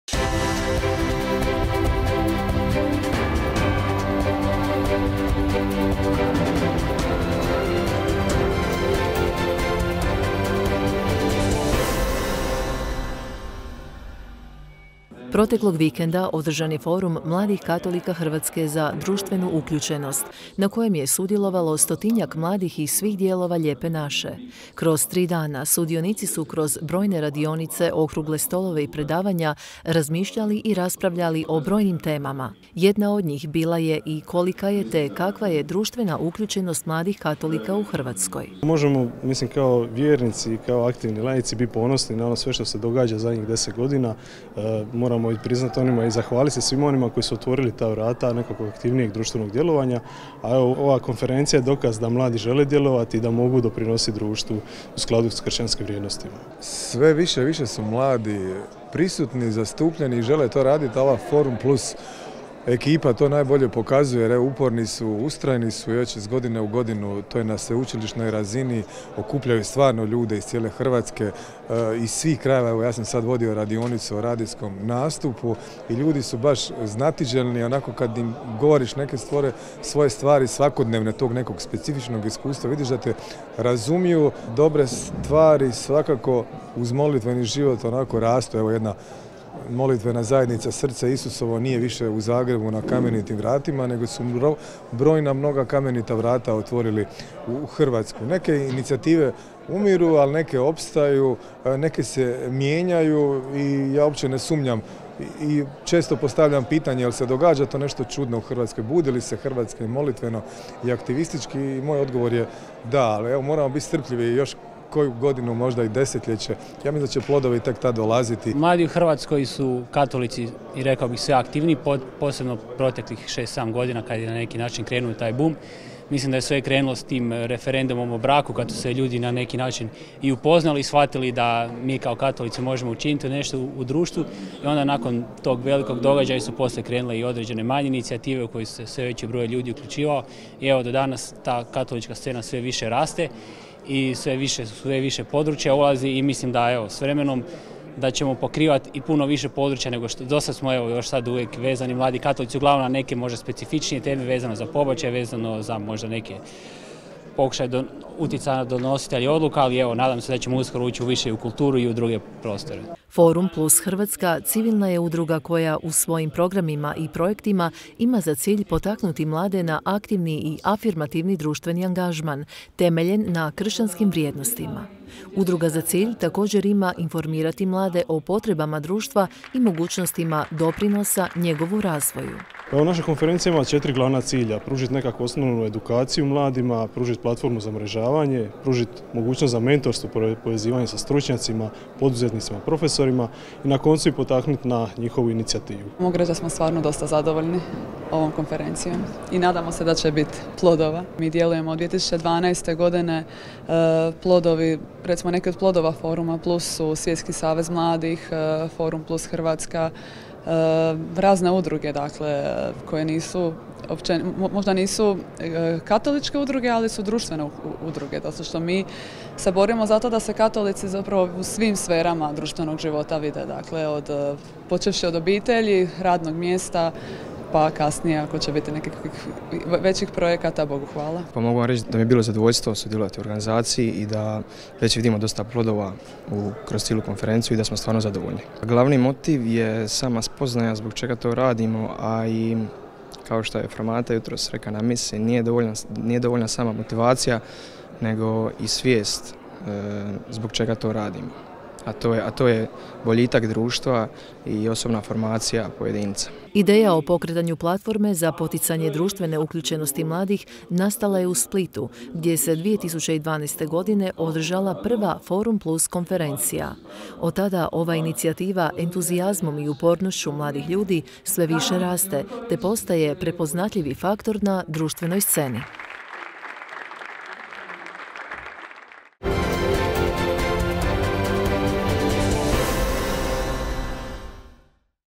Izdvojeno 2.3.2020. – Održan Forum mladih katolika Hrvatske za društvenu uključenost
Proteklog vikenda, održao se Forum mladih katolika Hrvatske za društvenu uključenost, na kojem je sudjelovalo 100-tinjak mladih iz svih djelovah Lijepe naše. Kroz tri dana, sudionici su kroz brojne radionice, okrugle stolove i predavanja razmišljali i raspravljali o brojnim temama. Jedna od njih bila je ti to, kolika je i kakva je društvena uključenost mladih katolika u Hrvatskoj.